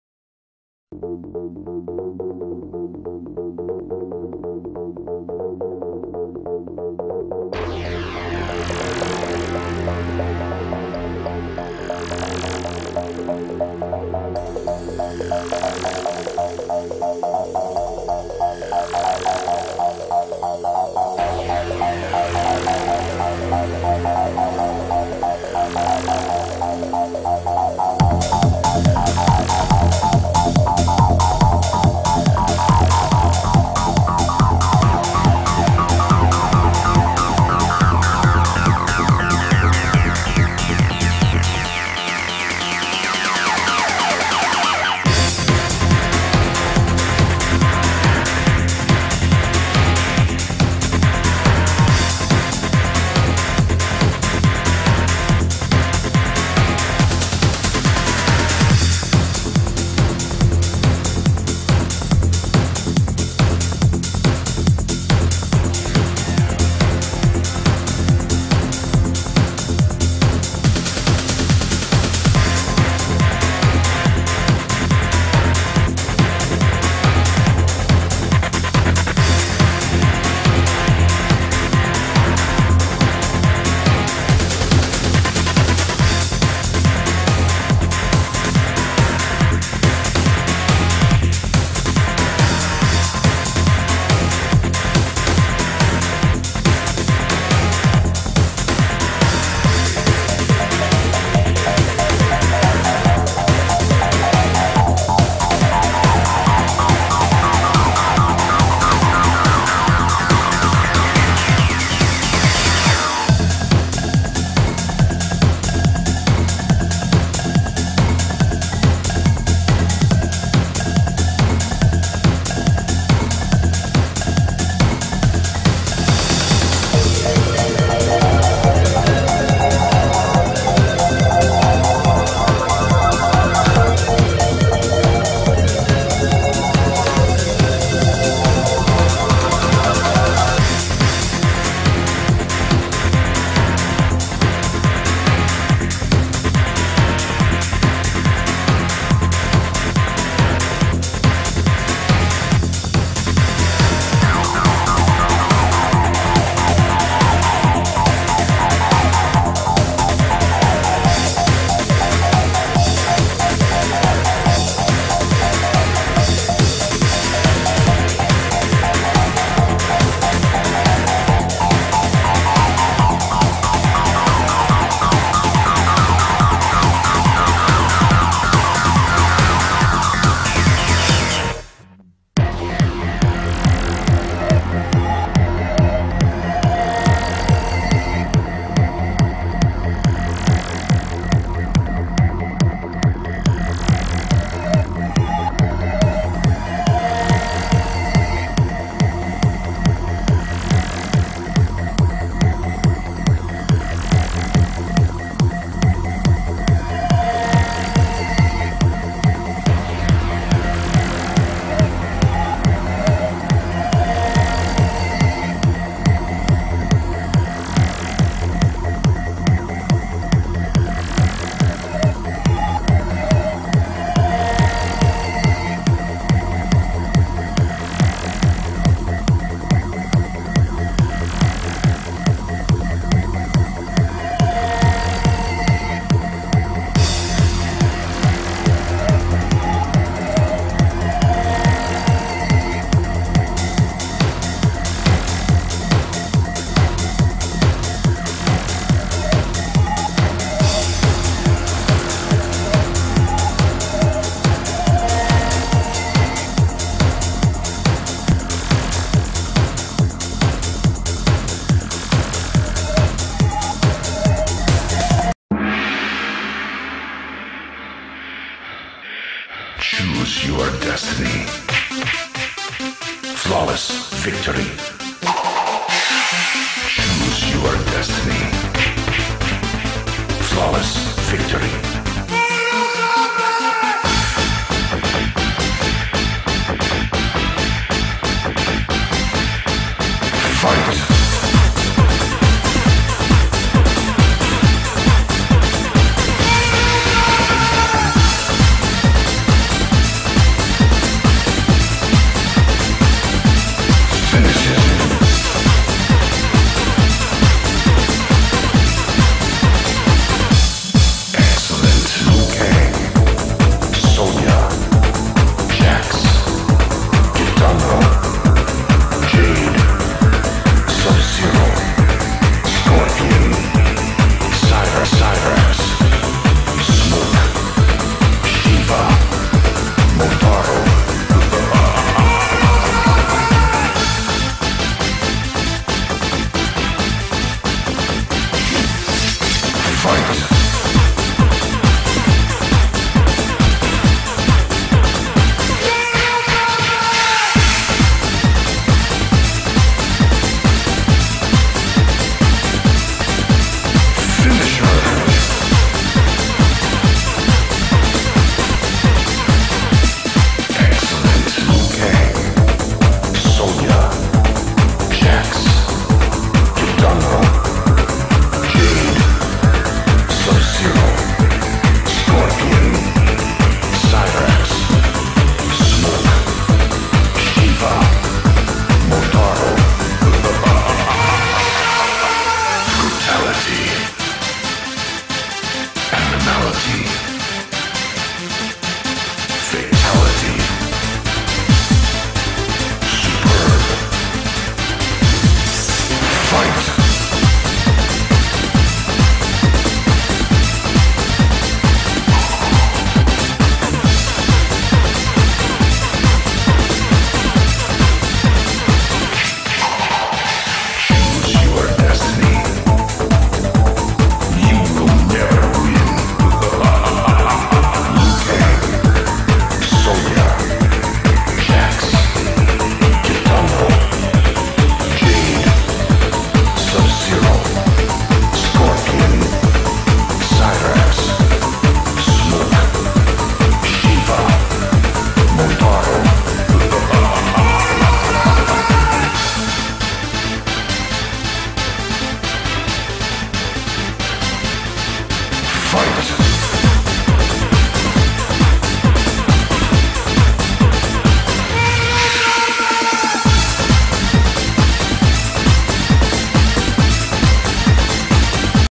Mortal kombat 2011 (MK9) NEW MK9 Sonya Pics and Screenshots along with TONS of MK9 Screenshots
Brand new MK9 Sonya sexy pictures and screenshots along with Kitana! Also tons of never before seen screenshots and pictures of all MK9 Characters so far! Mortal Kombat 2011
Mortal Kombat 9! (MK9) Slideshow all done and put together by COLT!